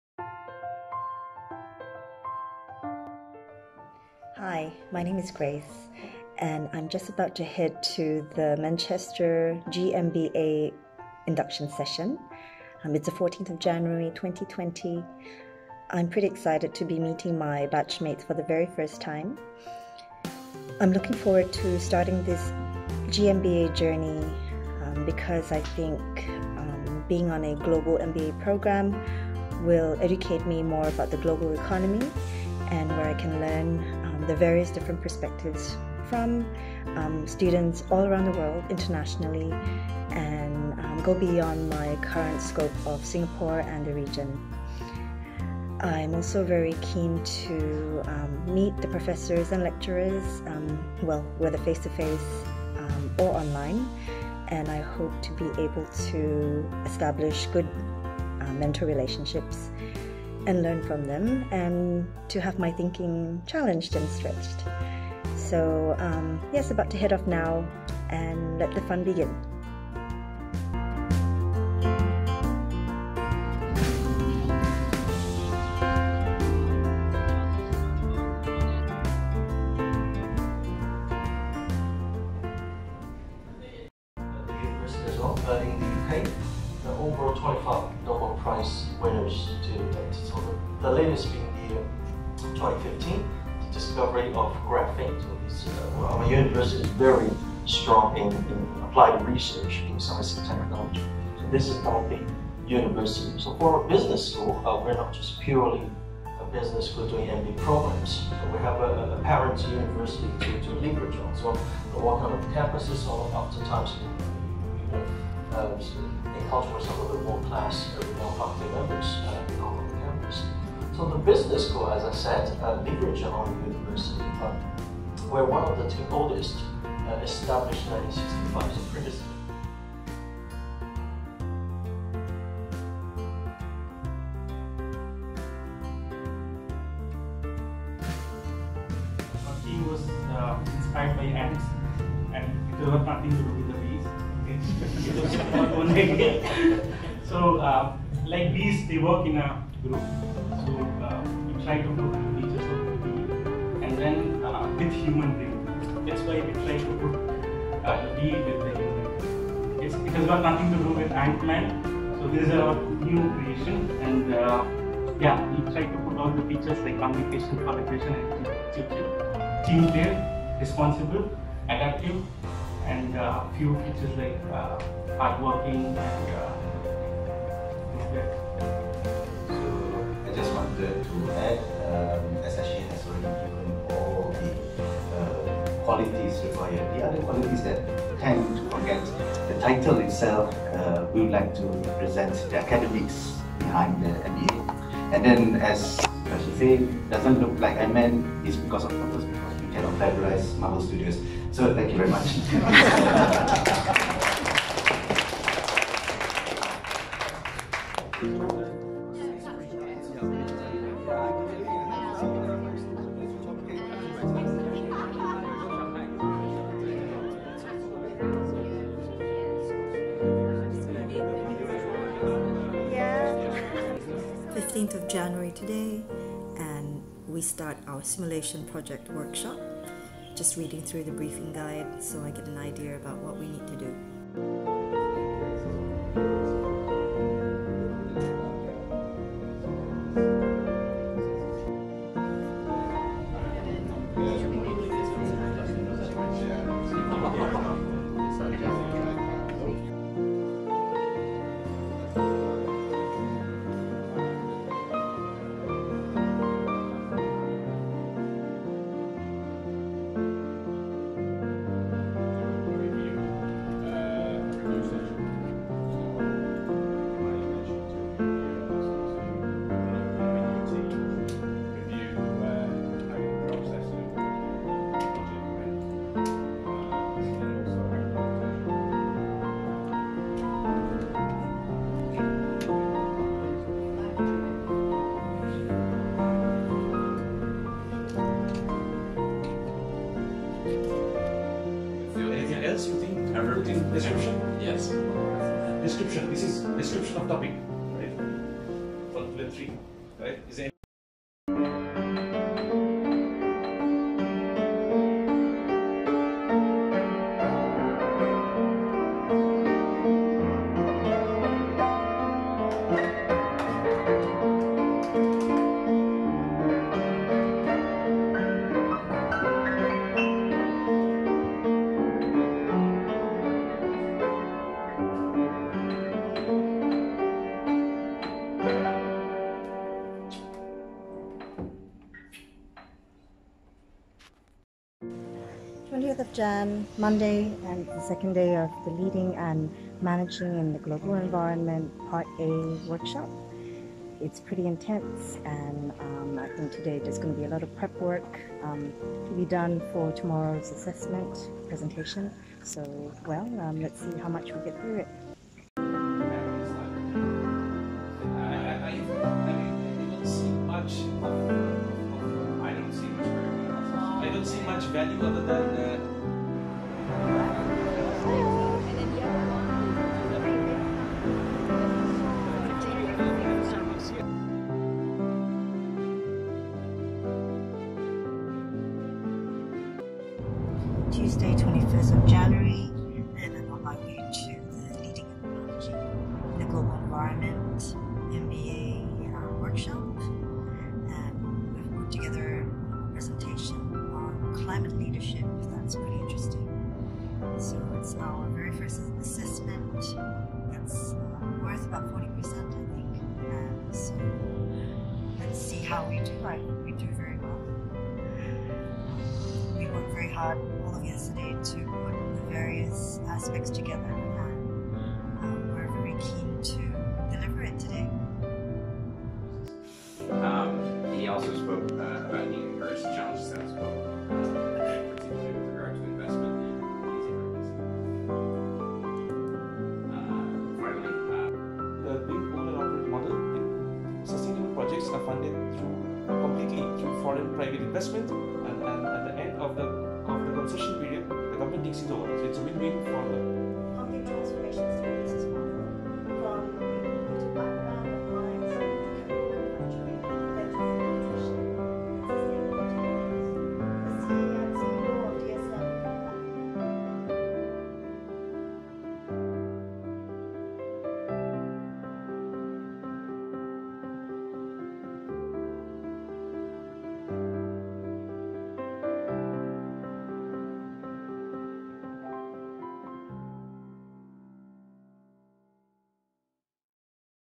Day in a Life Vlog 4 | Grace Loh (Singapore)
Welcome to the 'Day in a Life of a Manchester Global MBA Student' vlog series! In this fourth vlog, we follow Grace Loh as she takes you along a typical day in a life of a part-time MBA student in Singapore. Enjoy!